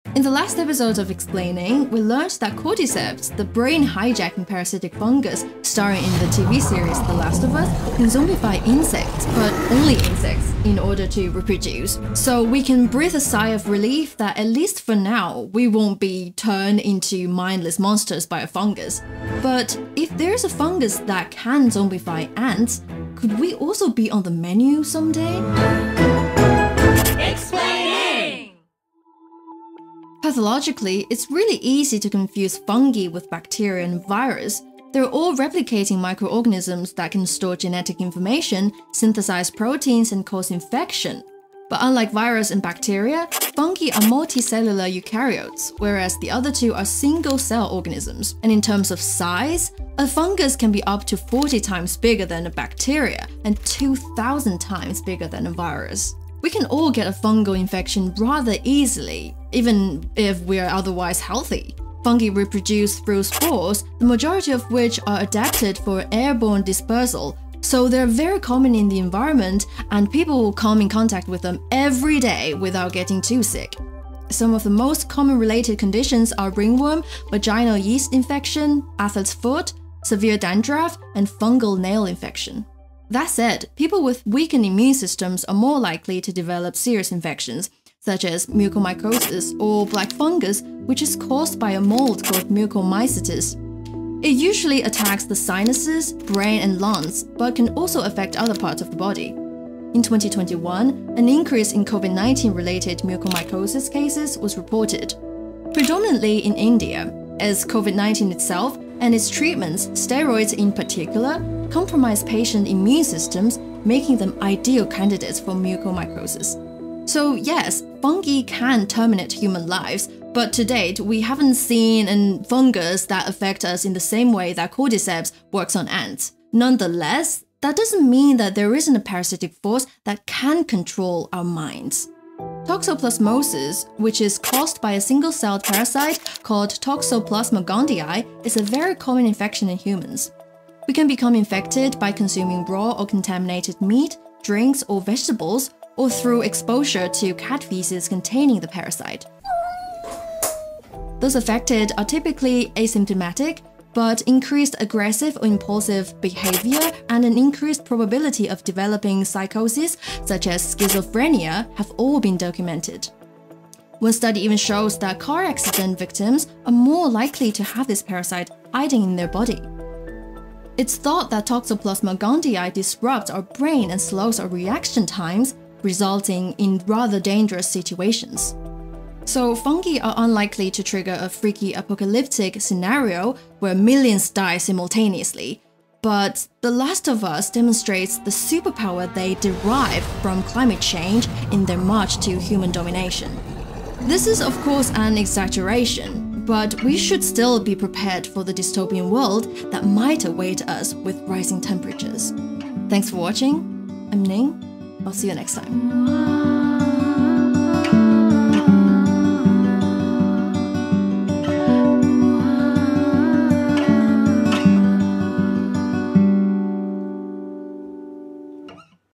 The Last of Us: Things about fungi from A to Zombie
In the last episode of explaiNing, we talked about the zombifying fungus cordyceps featured in thelastofus is actually edible in real life, and we know for sure that it only zombifies insects and not humans. But, what exactly is a fungus? And is it possible that one day, there will be some sort of parasitic force that turns us into mindless monsters?